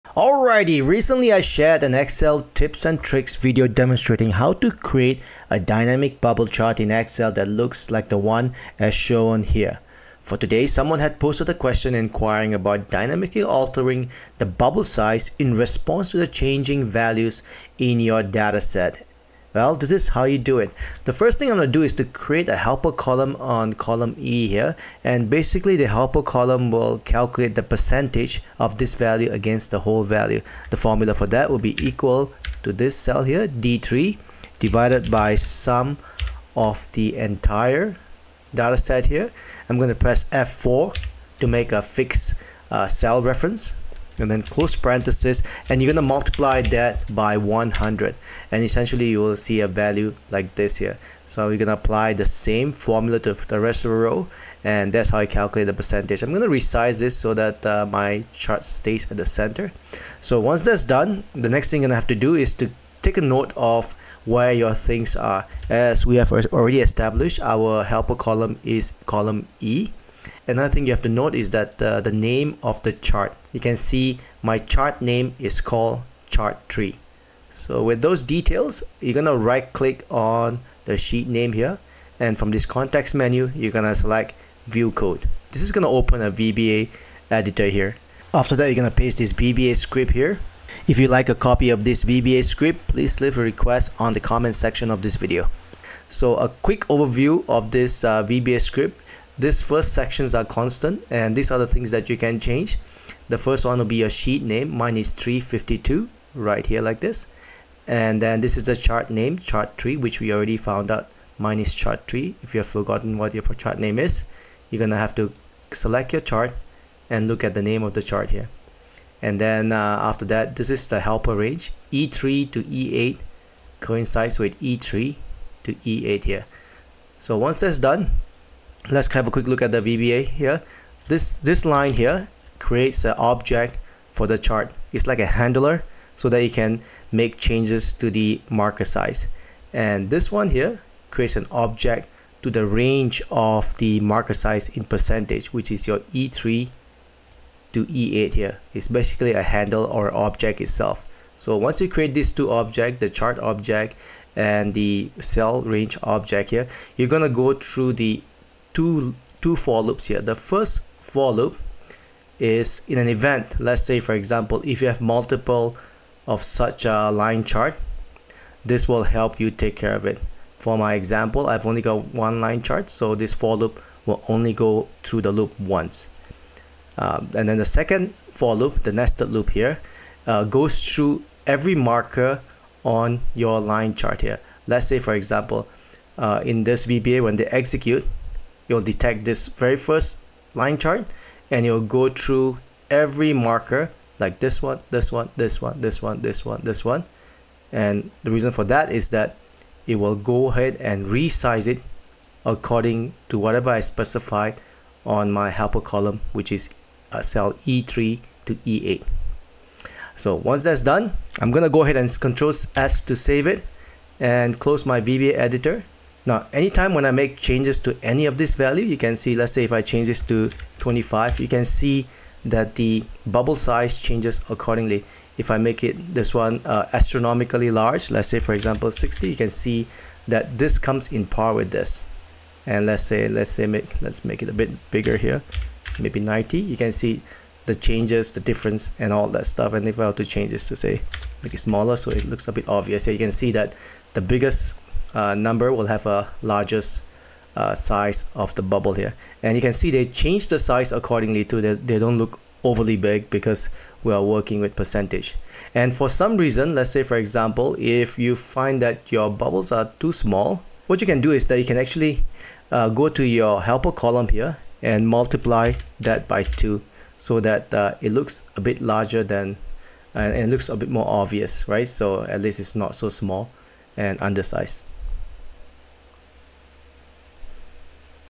Dynamically change the bubble size in a bubble chart in Excel - Excel Tips and Tricks
Learn how to dynamically change the bubble size in the bubble chart in Excel.

In Excel, adjusting the bubble size in a bubble chart is a straightforward process. To change the bubble size, click on the chart, go to the "Chart Design" tab, and select "Select Data." From there, choose the series for which you want to modify the bubble size and click "Edit." In the "Edit Series" dialog box, you can adjust the bubble size by modifying the values in the "Bubble Size" field. The size of a bubble in a bubble chart typically represents a third data dimension, often indicating a quantitative value associated with each data point. To resize bubbles, you can either modify the data directly or use the formatting options available in Excel. Customizing a bubble chart involves various aspects, such as changing colors, adding data labels, and adjusting axis titles. To further customize the chart, right-click on different elements, like the bubbles or the axis labels, and explore the formatting options. Keep in mind that the size of a bubble in a bubble chart is crucial as it visually communicates the magnitude of the associated data point, providing an additional layer of information beyond the x and y axes.

Helper Column
 =D3/SUM($D$3:$D$8)*100*2

Dynamic Bubble Sizing
1) Right-click on sheet.
2) View Code
3) Paste the VBA script